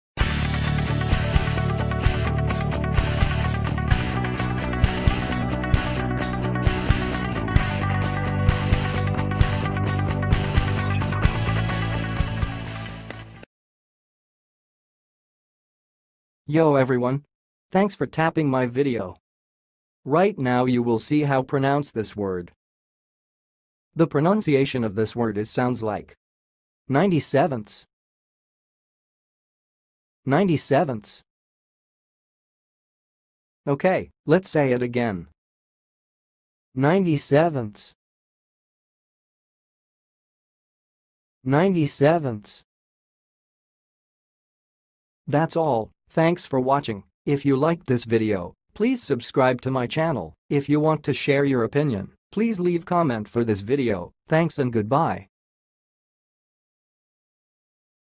NINETY-SEVENTHS - How to pronounce it?
NINETY-SEVENTHS - Ninety-sevenths: What it is and how to pronounce it correctly in 2023

Ninety-sevenths refers to the ordinal number form of the fraction 97/100.
In this form, the number 97 represents the numerator, and 100 represents the denominator.
This indicates that something is divided into 100 equal parts, and we are referring to the 97th part.

Pronunciation:
To pronounce 'ninety-sevenths', you can say: 'nine-tee sev-uhnthz' or 'nine-tee sev-uhnth-s'.

Test yourself by speaking the following examples:
1. Ninety-sevenths of the pizza was left untouched
2. He scored ninety-sevenths on the math test
3. Only ninety-sevenths of the cake remained after the party
4. The company's shares declined by ninety-sevenths
5. She had to divide the apples into ninety-sevenths